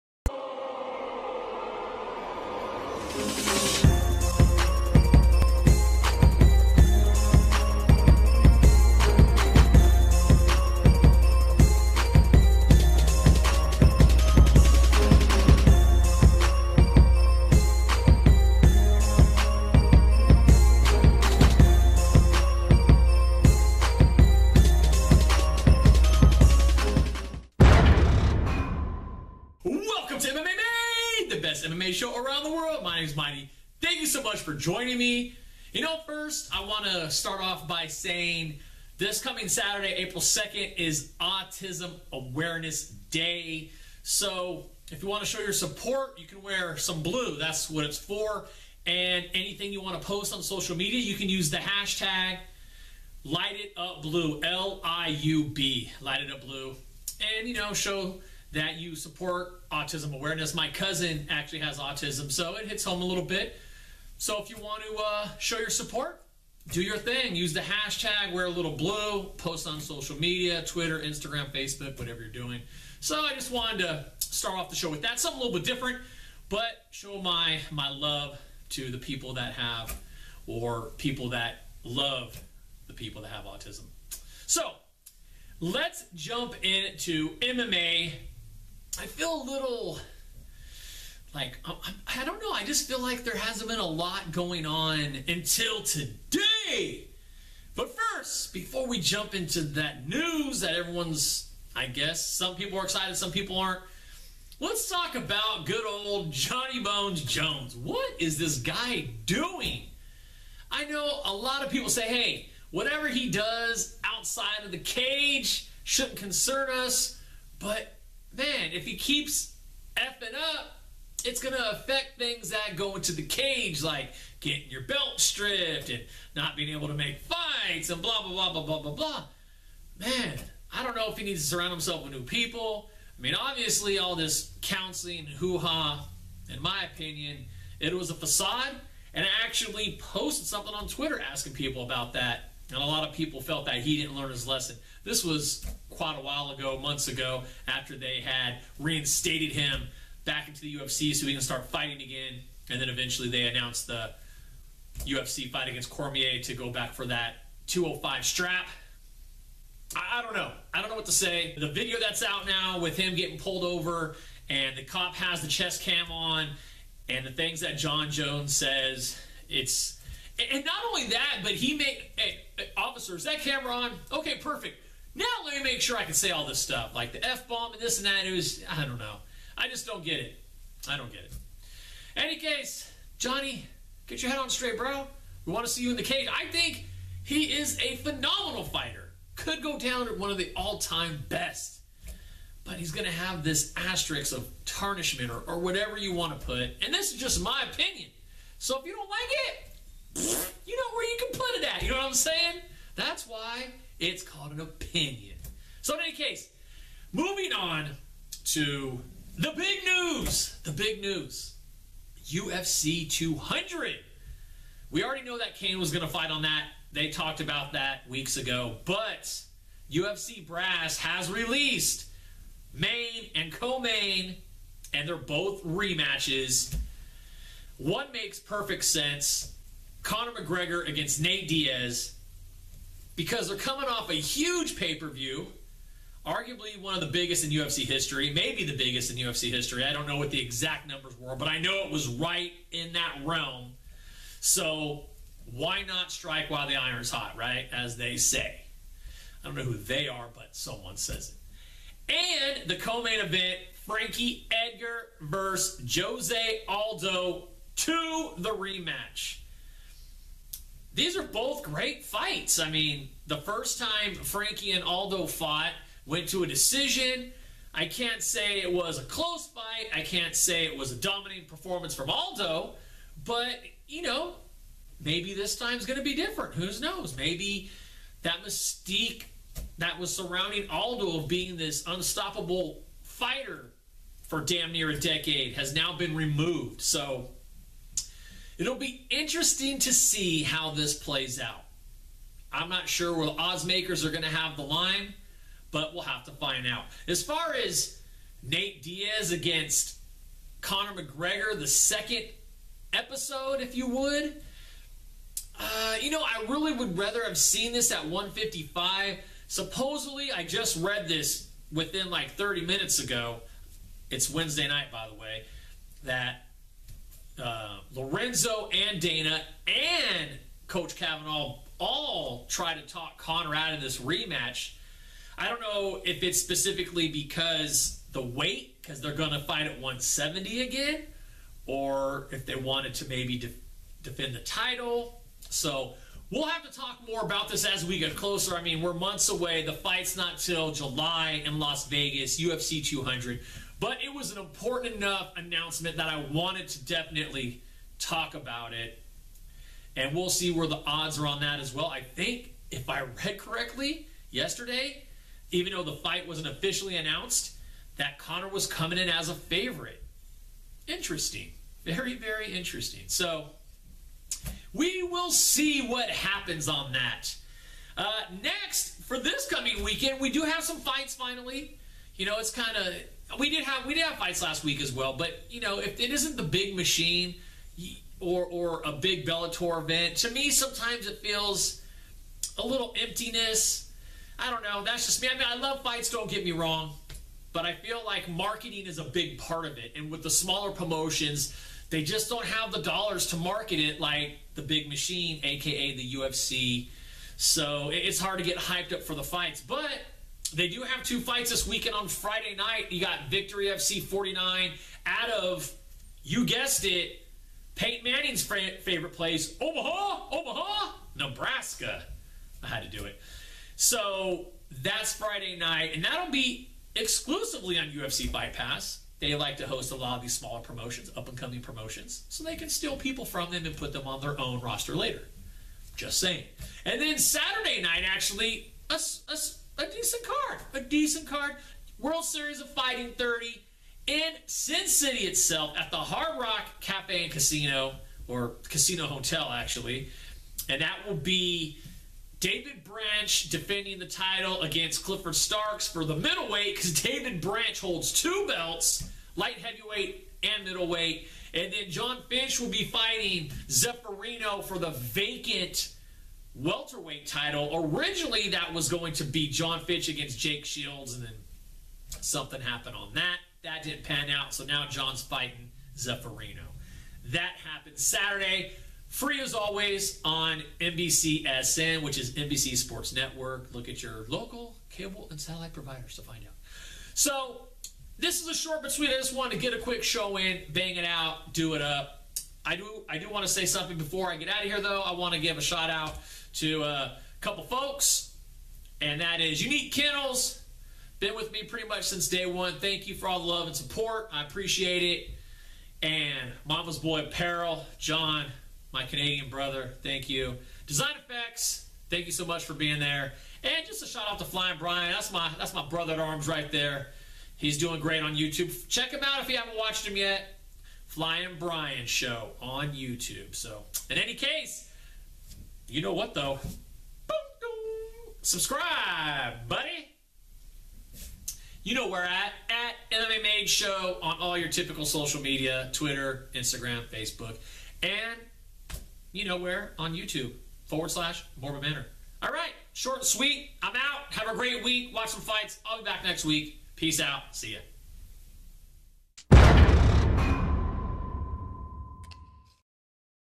UFC 200 gets main & co-main, Jon Jones violates probabtion
The big news this week is UFC announcing the main and co-main events for UFC 200 and Jon Jones back in the public eye.